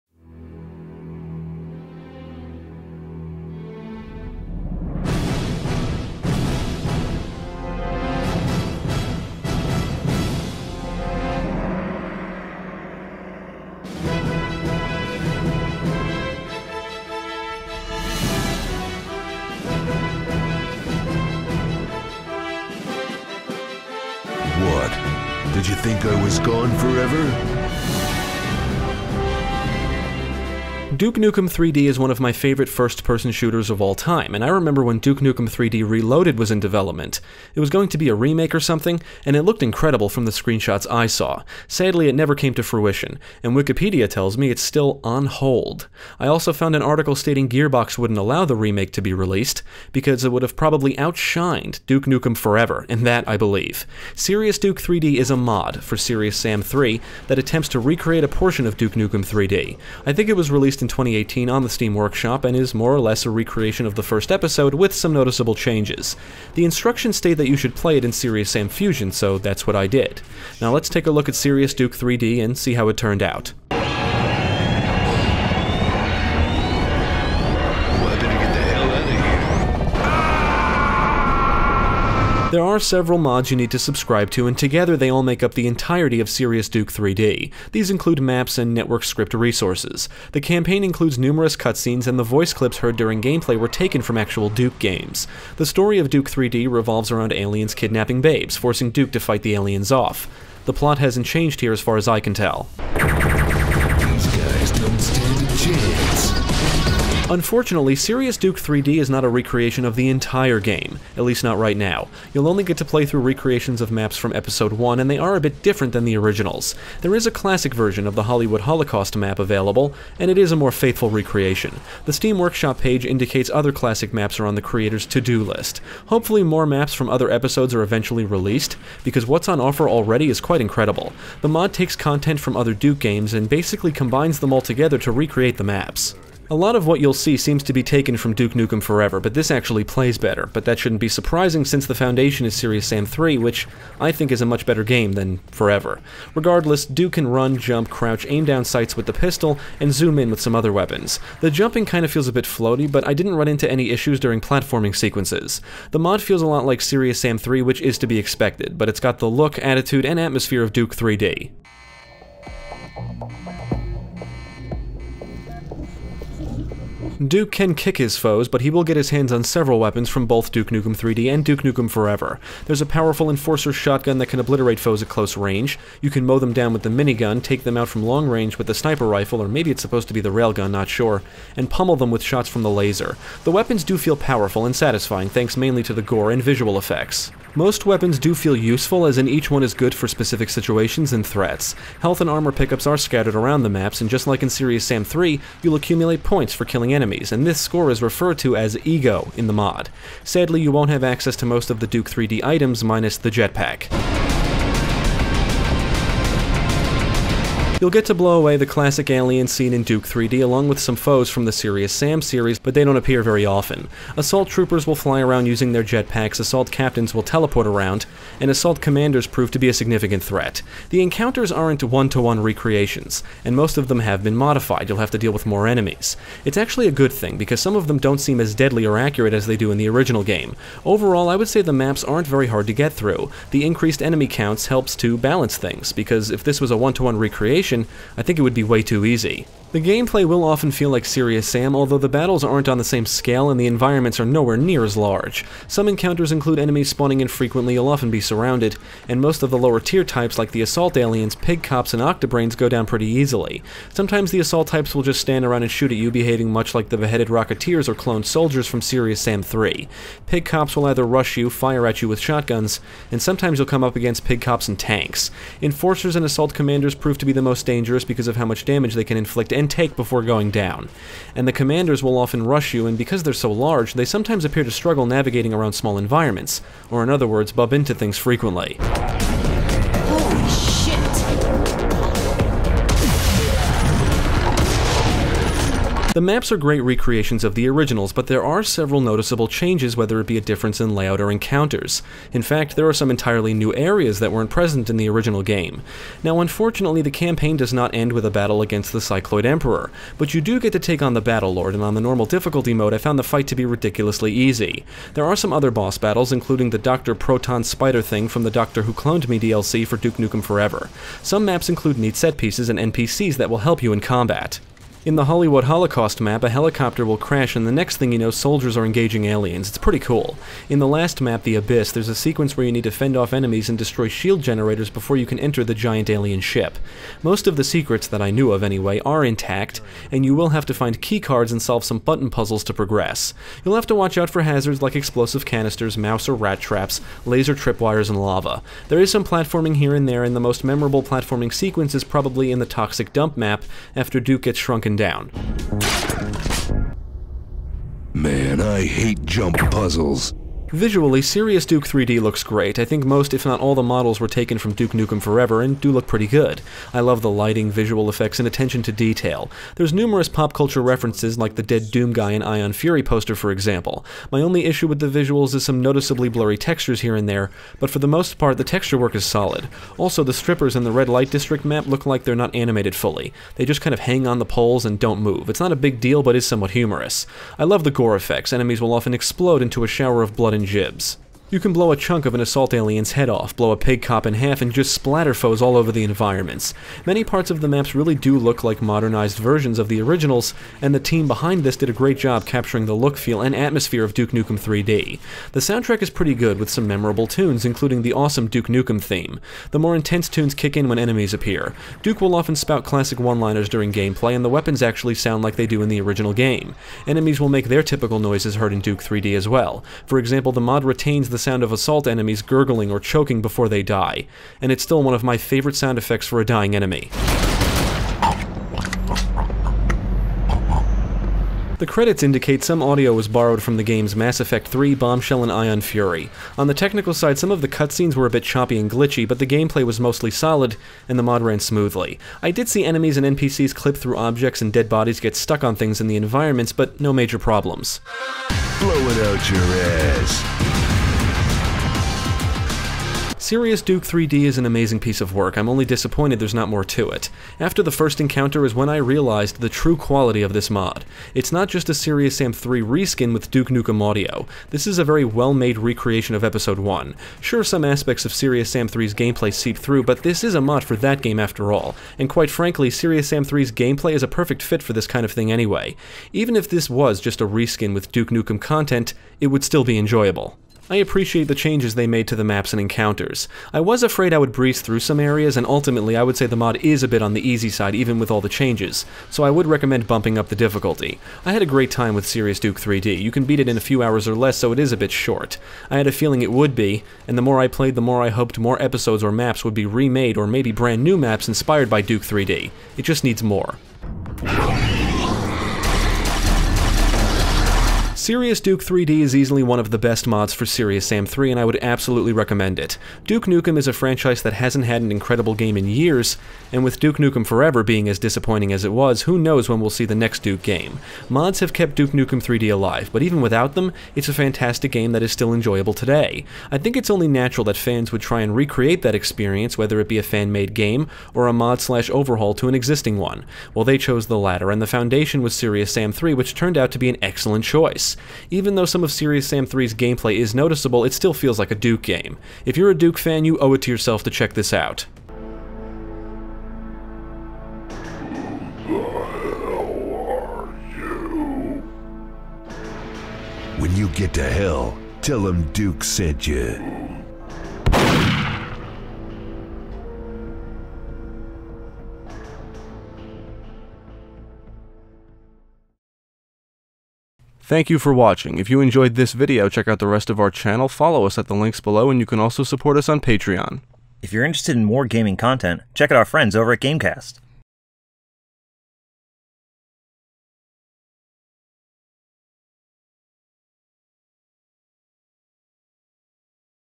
Serious Duke 3D Video Review
Gaming Pastime reviews Serious Duke 3D, a mod or group of mods for Serious Sam 3.

Serious Duke 3D is a mod for Serious Sam 3 that recreates a chunk of levels from Duke Nukem 3D and the result is phenomenal.